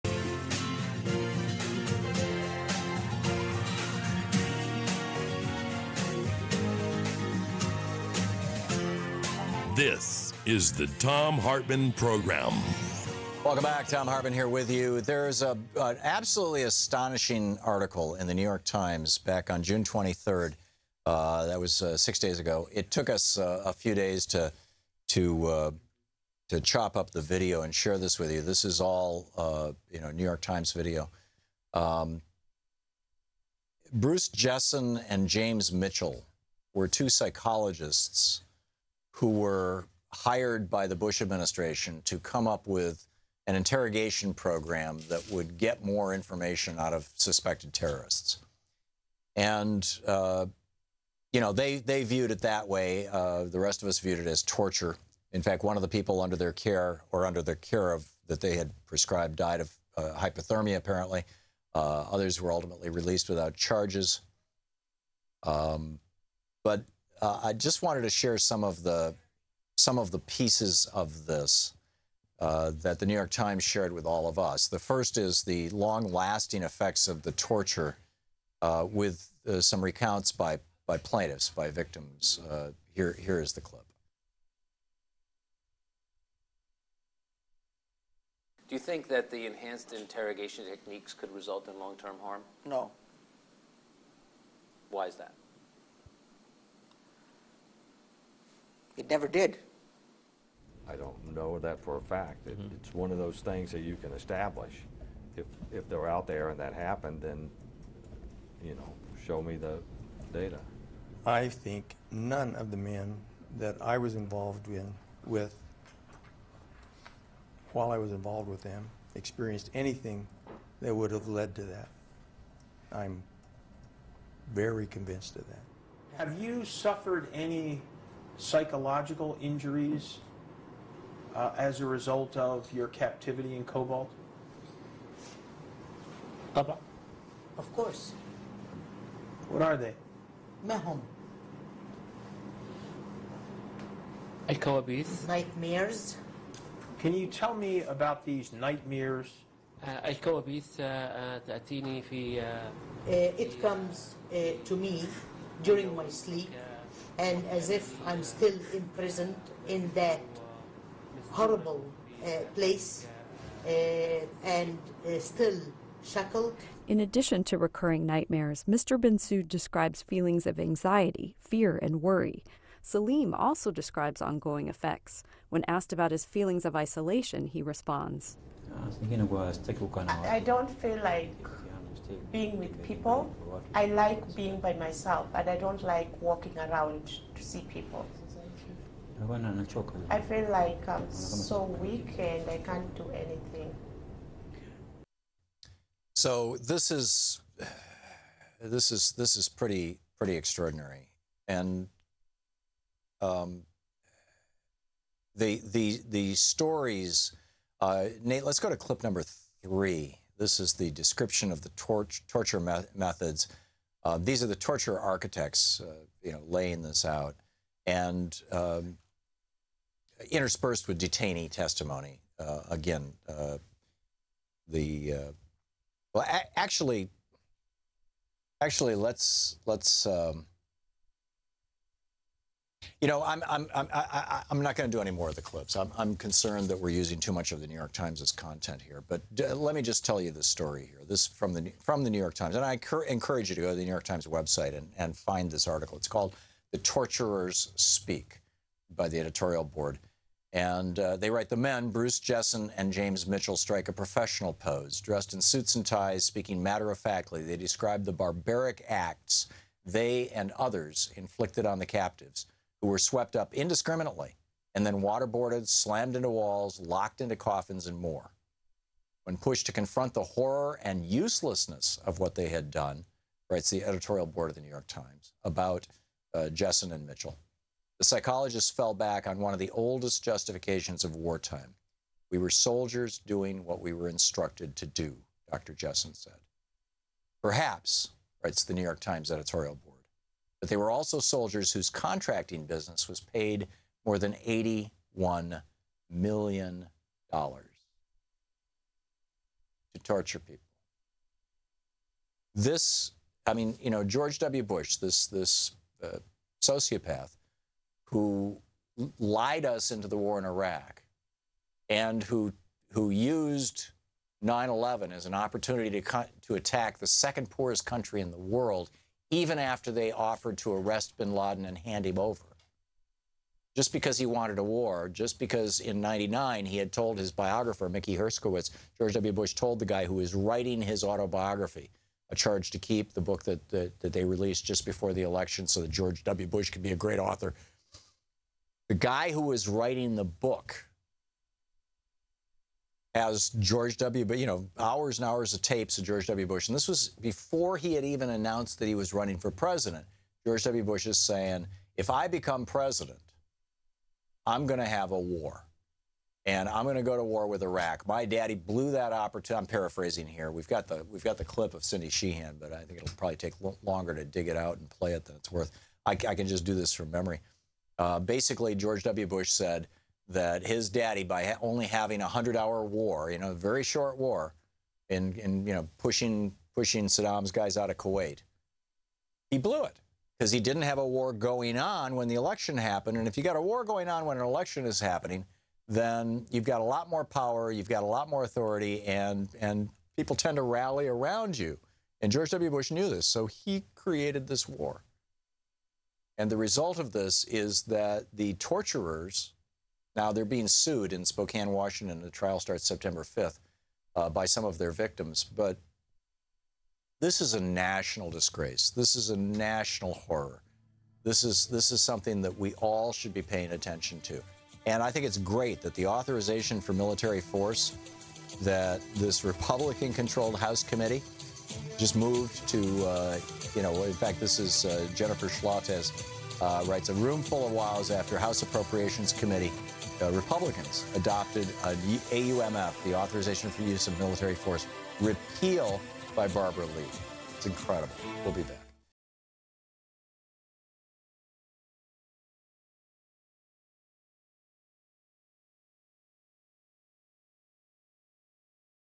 Meet the Men Behind the CIA Detainee Torture Program After 9/11, Being Sued by Former Detainees
Thom covers the story of 2 men responsible for designing the CIA Counterterrorism unit's "enhanced interrogation techniques" and who are now being sued by former detainees for the treatment they suffered while in custody.